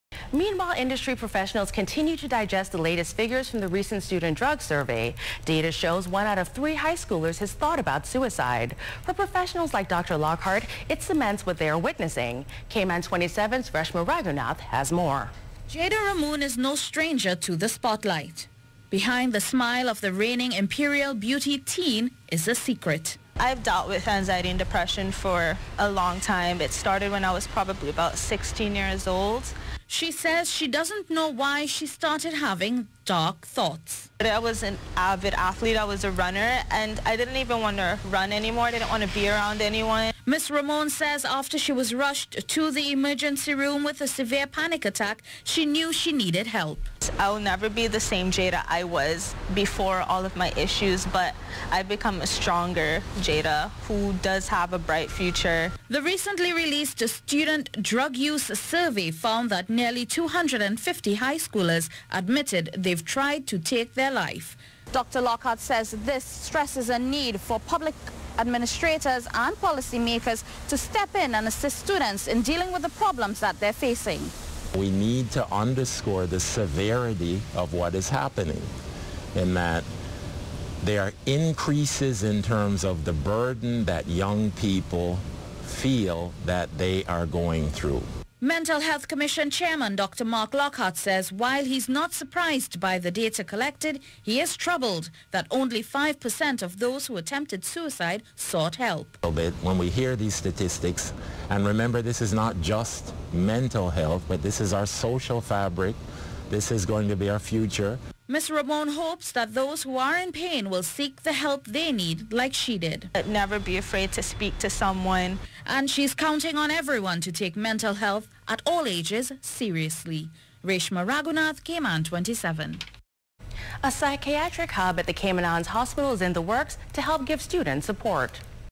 Spotlight on mental health: Student survey highlights concerns
Industry professionals continue to digest the last figures from the recent student drug survey.
Data shows one out of three high schoolers has thought about suicide.
For professionals like Dr. Marc Lockhart, it cements what they are witnessing.
Jada Ramoon is no stranger to the spotlight.
Behind the smile of the reigning Imperial Beauty teen, has a secret.

"I have dealt with anxiety and depression for a long time. It started when I was about sixteen years old."

She said she doesn't know why she started having dark thoughts.

"I was an avid athlete, I was a runner and I didn't want to even run anymore, I did not want to be around anyone," said Ms. Ramoon.

She said after she was rushed to the emergency room with a severe panic attack, she knew she needed help.

"I will never be the same Jada I was before all of my issues, but I have become a stronger Jada who does have a bright future," she said.

The recently released student drug use survey found that nearly 250 high-schoolers admitted they have tried to take their life.
Dr. Lockhart said this stresses a need for public administrators and policymakers to step in and assist students in dealing with the problems they are facing.

"We need to underscore the severity of what is happening, in that, there are increases in terms of the burden that young people feel that they are going through," Mental Health Commission chairman Dr. Marc Lockhart said.

While he is not surprised by the data collected, he is troubled that only 5 percent of those who attempted suicide sought help.

"When we hear these statistics and remember this is not just mental health this is our social fabric, this is going to be our future," said

Ms. Ramoon hopes that those who are in pain will seek the help they need, like she did.

"Never be afraid to speak to someone," she said.
And she's counting on everyone to take mental health at all ages seriously.

A psychiatric hub at the Cayman Islands hospital is in the works to help give students support.